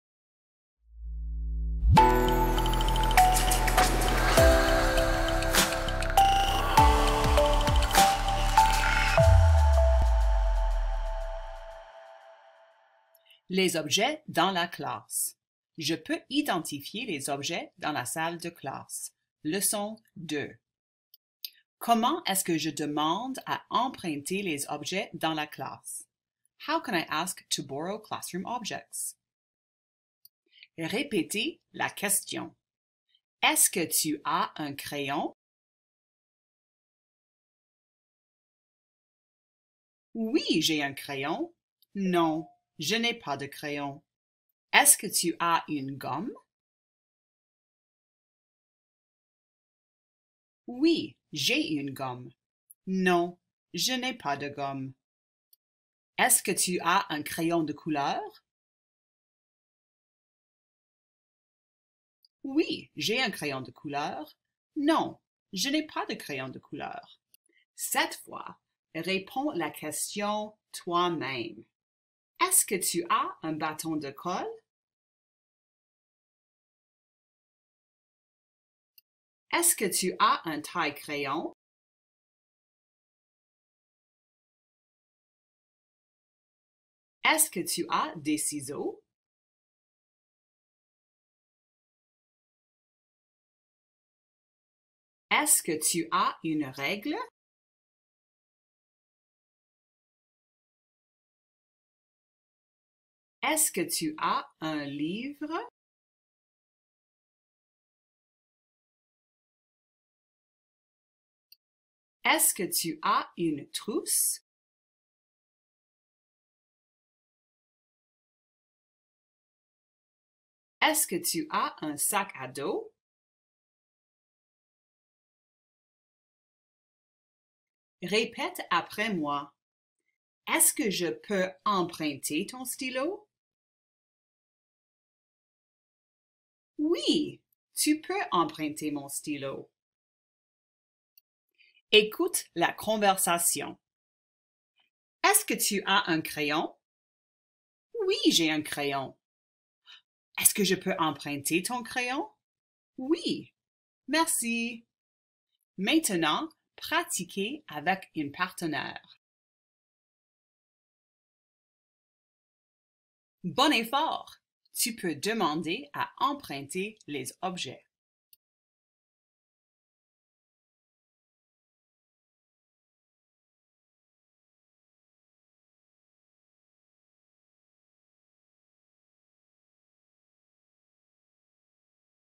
Grade 5 Week 3 Emprunter des objets
Dans cette vidéo, les élèves apprendront comment demander à emprunter diverses fournitures scolaires.

In this video, students will learn how to ask to borrow various school supplies.